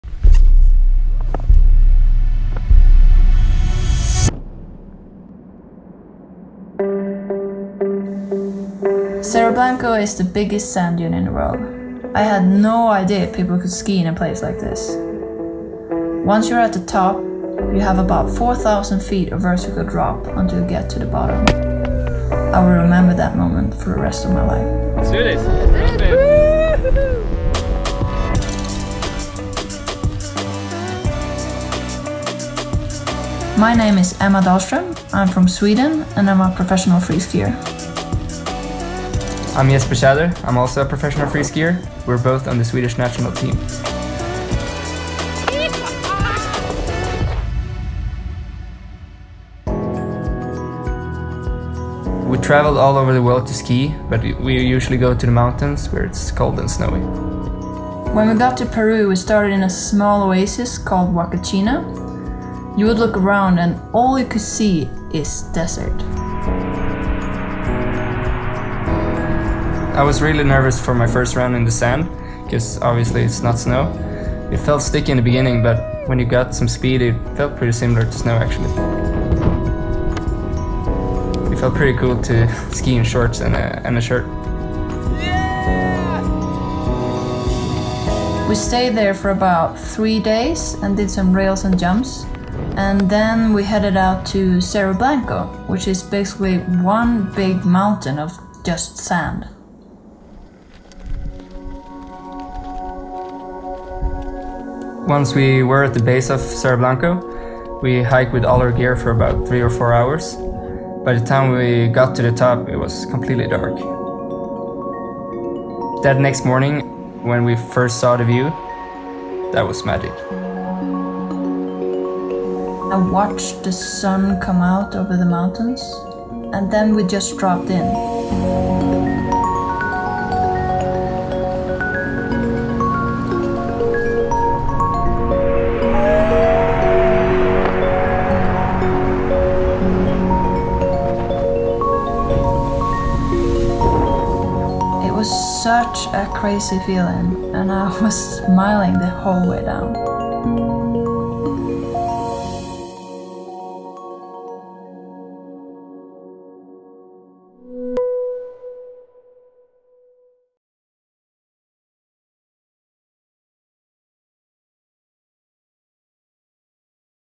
Shredding the World's Highest Sand Dunes on Skis
Emma Dahlström and Jesper Tjäder are both professional free-skiers who compete on the Swedish National Ski Team. In their careers, the pair has travelled around the world to ski some of the steepest, iciest and gnarliest slopes. But they had never experienced anything like the sheer vertical madness they took on during a recent trip to Peru. There, they shredded the sandy hillsides of Huacachina and Cerro Blanco—the highest sand dune in the world.